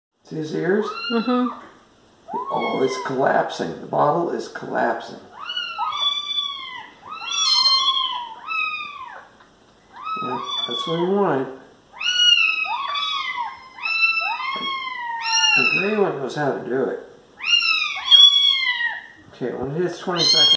Bottle Fed Feral Kittens
We had to bottle feed these feral kittens because their mom died.  We brought them indoors and raised them.  Best cats ever! ... Carl, Mike, Johnny and Cocoa.